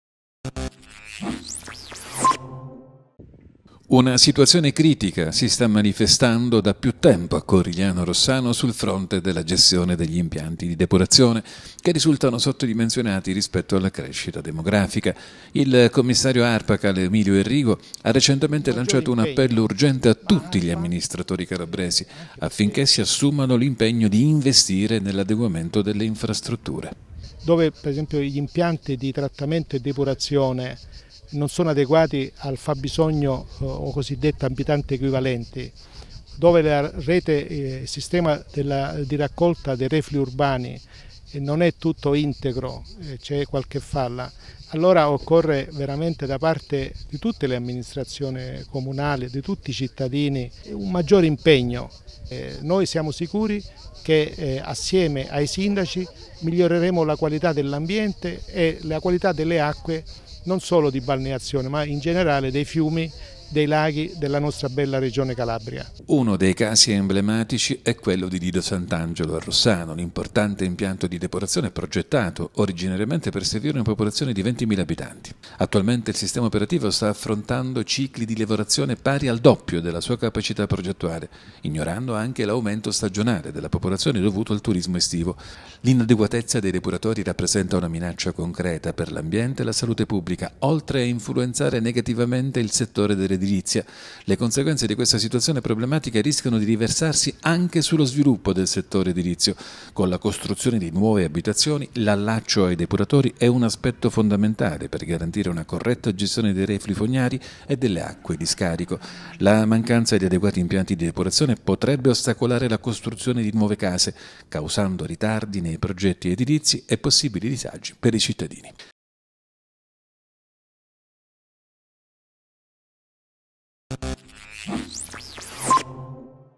Emergenza depuratori a Corigliano Rossano: sono sottodimensionato, a rischio anche l'edilizia
Corigliano Rossano si trova di fronte a un'urgenza: impianti di depurazione sottodimensionati rispetto alla crescita demografica. Il commissario di Arpacal, Emilio Errigo, lancia un appello a tutti i sindaci calabresi di adeguare le infrastrutture.